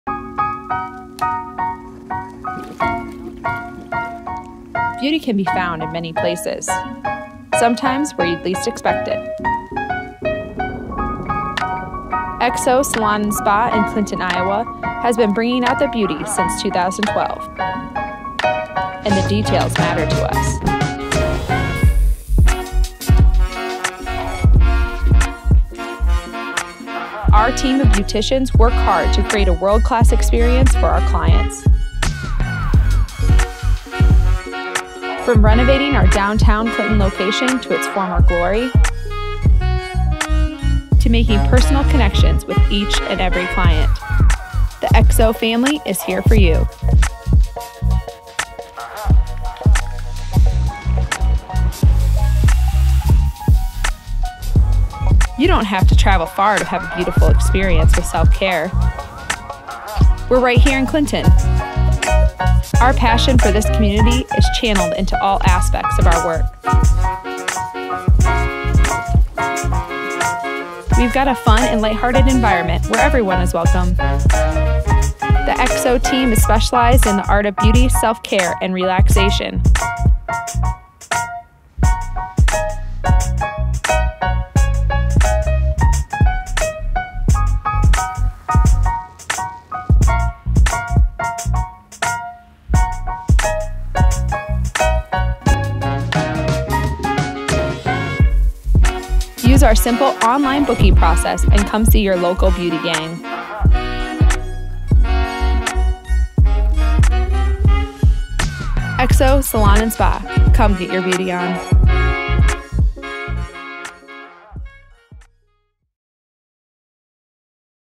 Promotional Video for Exo Salon & Spa | Clinton, IA
It was a blast working with Kate & the wonderful staff at Exo Salon & Spa to create this video! Book yourself an appointment and enjoy a wide variety of beauty & self-care services, and to check out the work they've done renovating their building to it's former glory!